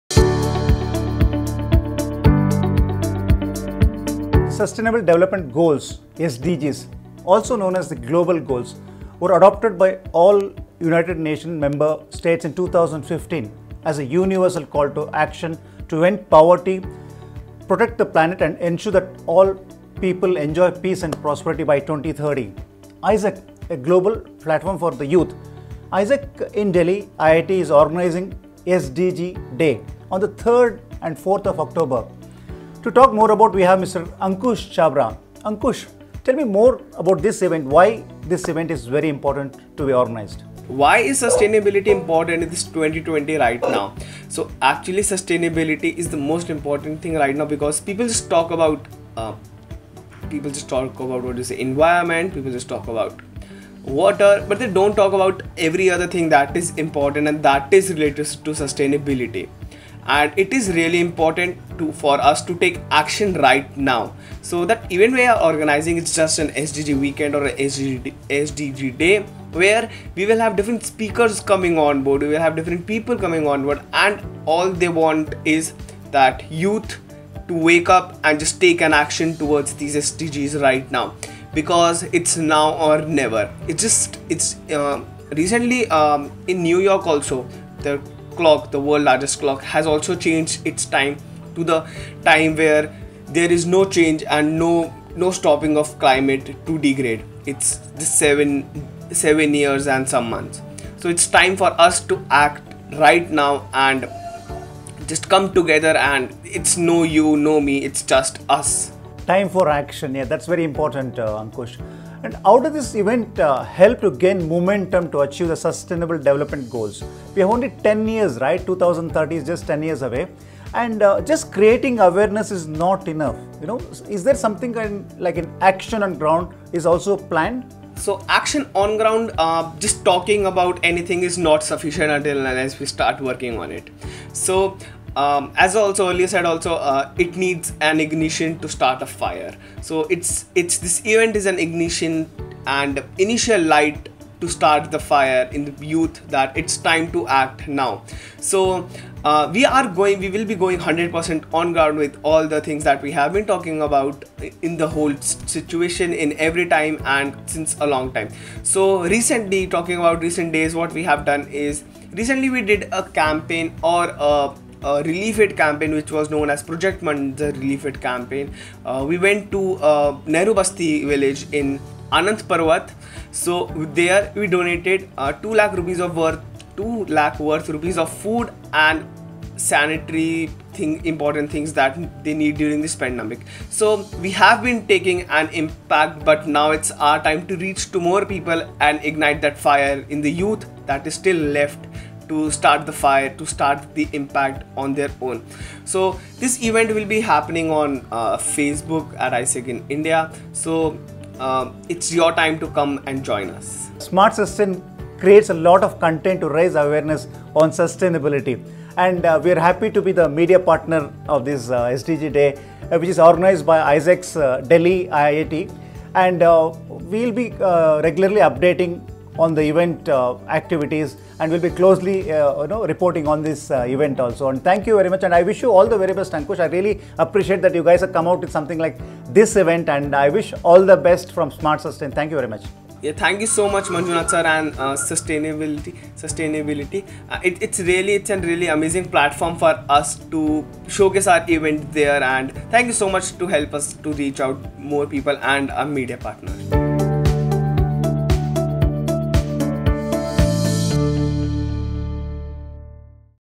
SUSTAINABLE DEVELOPMENT GOALS - AIESEC in DELHI IIT TEAMS UP WITH SMART SUSTAIN
Achieving Sustainable Development Goals (SDG) s is the need of the hour and gaining momentum amidst the challenges posed by the pandemic would seem impossible. But that is not the case, AIESEC proves by conducting their SDG day activities. The quick and vigorous actions to deal with the pandemic and achieving the SDGs seem to go hand in hand when it comes to knowing about the actions of AIESEC, though it is a challenge indeed. With enhanced political will and commitment, the world can achieve what is needed for the global community.